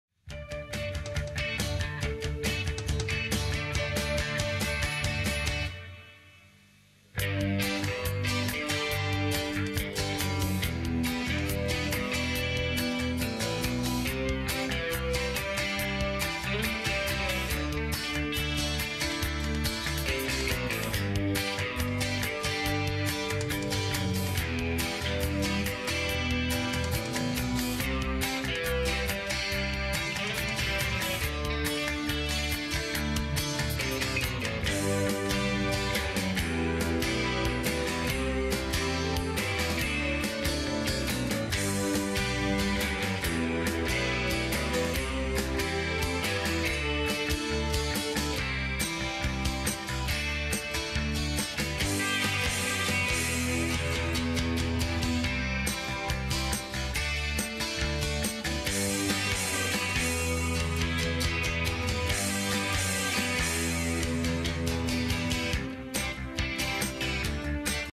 Lake Region vs Inver Hills Region Championship prelude
The 1981-82 Lake Region Royals were truly a special team made up of all North Dakota players. They won the MonDak conference, state and regional tournaments and advanced to the national tournament.